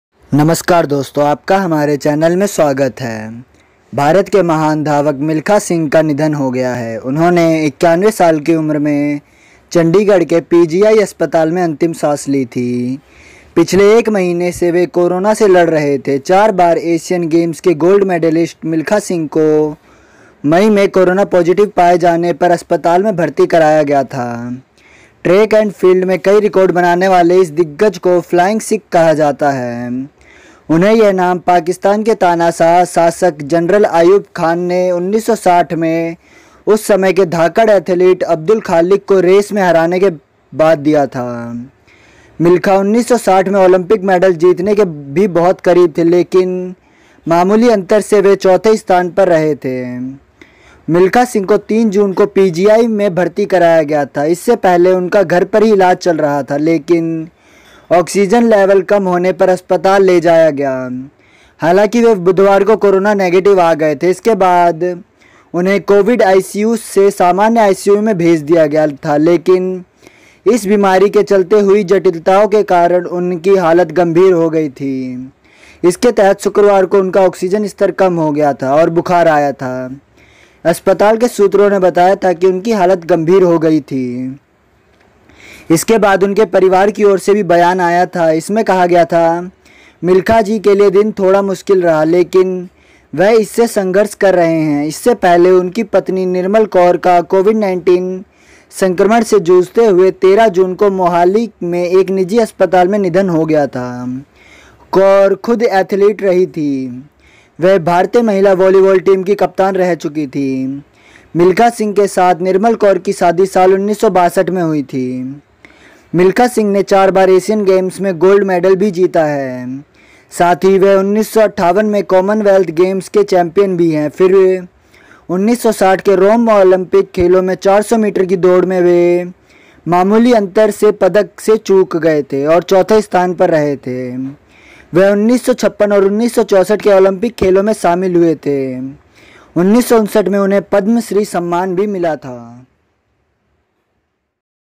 ब्रेकिंग: मिल्खा सिंह का निधन, 91 साल की उम्र में कोरोना के चलते फ्लाइंग सिख ने ली अंतिम सांस
भारत के महान धावक मिल्खा सिंह का निधन हो गया है।

उन्होंने 91 साल की उम्र में चंडीगढ़ के पीजीआई अस्पताल में अंतिम सांस ली।

पिछले एक महीने से वे कोरोना से लड़ रहे थे।

मिल्खा सिंह को 3 जून को पीजीआई में भर्ती कराया गया था।

इससे पहले उनका घर पर ही इलाज चल रहा था लेकिन ऑक्सीजन लेवल कम होने पर अस्पताल ले जाया गया।

हालांकि वे बुधवार को कोरोना नेगेटिव आ गए थे।

इसके बाद उन्हें कोविड आईसीयू से सामान्य आईसीयू में भेज दिया गया था।

लेकिन इस बीमारी के चलते हुई जटिलताओं के कारण उनकी हालत गंभीर हो गई थी।

इसके तहते शुक्रवार को उनका ऑक्सीजन स्तर कम हो गया था और बुखार आया था।

अस्पताल के सूत्रों ने बताया था कि उनकी हालत गंभीर हो गई थी।

इसके बाद उनके परिवार की ओर से भी बयान आया था. इसमें कहा गया था, 'मिल्खा जी के लिये दिन थोड़ा मुश्किल रहा।

लेकिन वह इससे संघर्ष कर रहे हैं.'इससे पहले उनकी पत्नी निर्मल कौर का कोविड-19 संक्रमण से जूझते हुए 13 जून को मोहाली में एक निजी अस्पताल में निधन हो गया था।

कौर खुद एथलीट रही थीं, वह भारतीय महिला वॉलीबॉल टीम की कप्तान रह चुकी थीं।

मिल्खा सिंह के साथ निर्मल कौर की शादी साल 1962 में हुई थी।